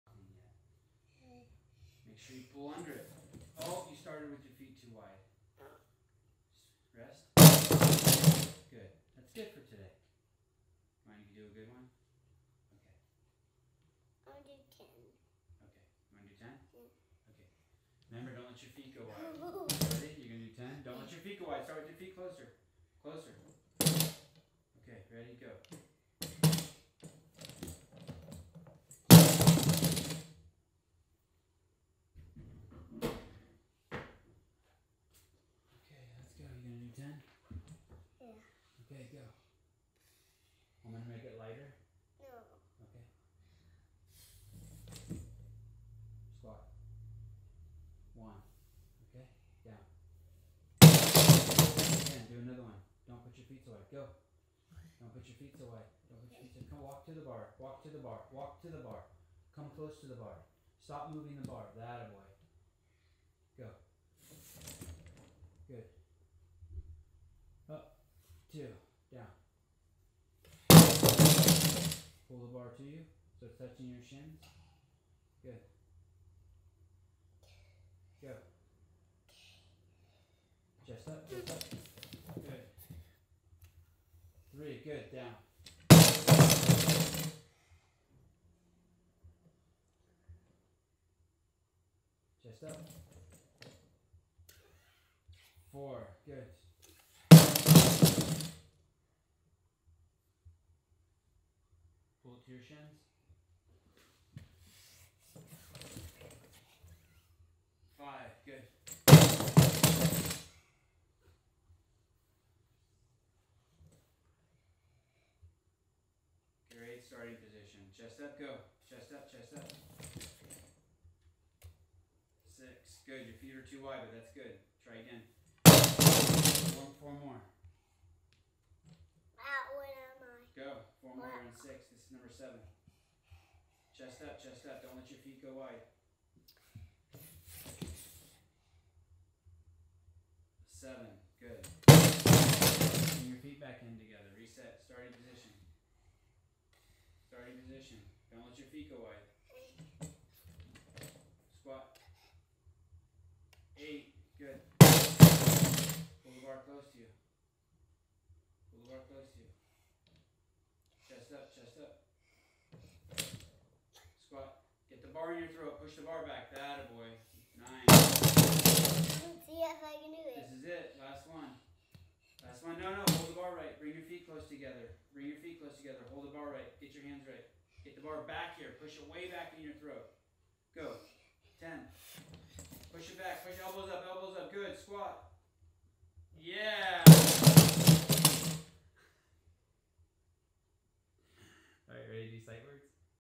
5 year old learning power clean and squat
5 year old learning power clean and squat